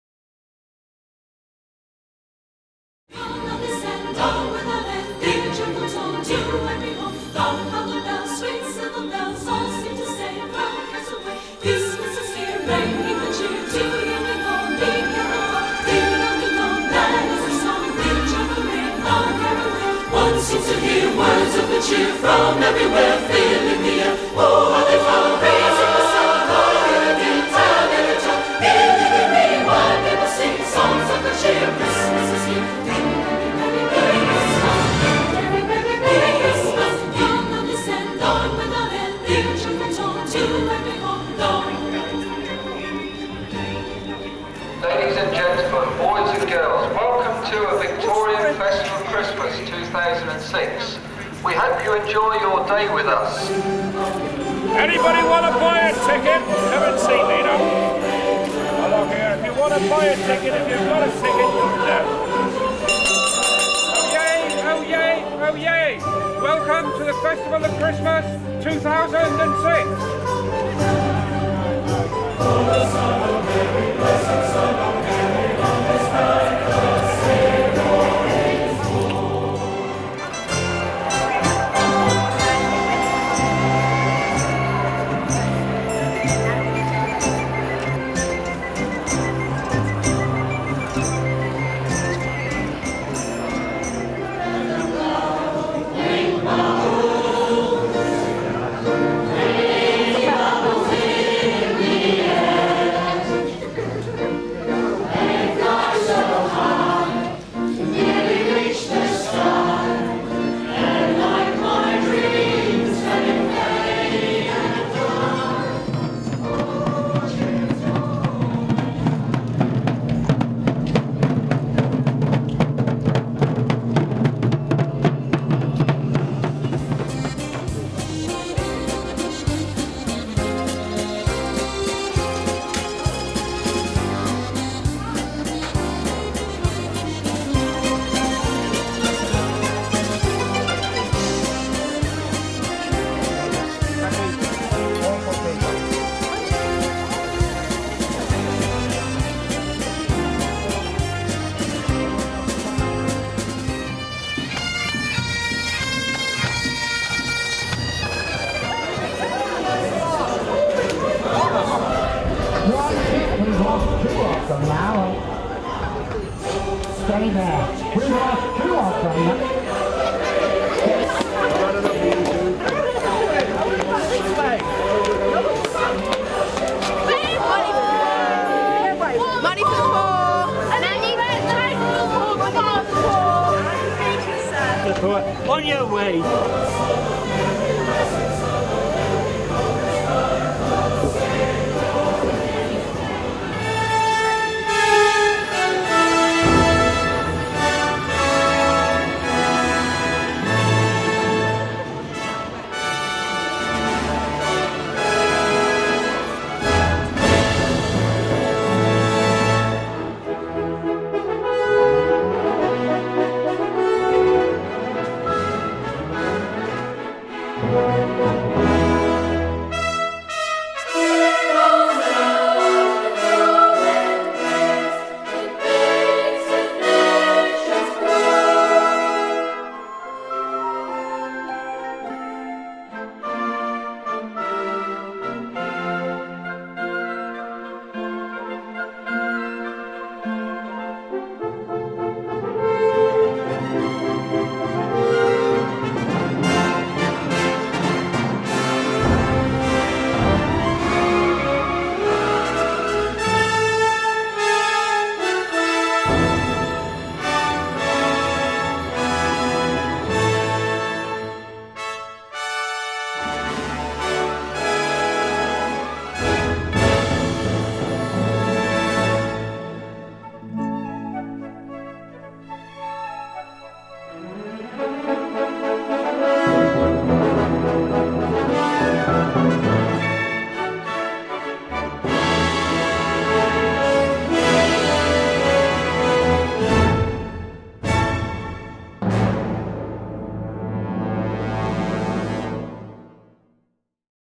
A Victorian Festival of Christmas 2006
The largest Christmas Festival on the South Coast returned for its seventh year, welcoming some 30,000 people.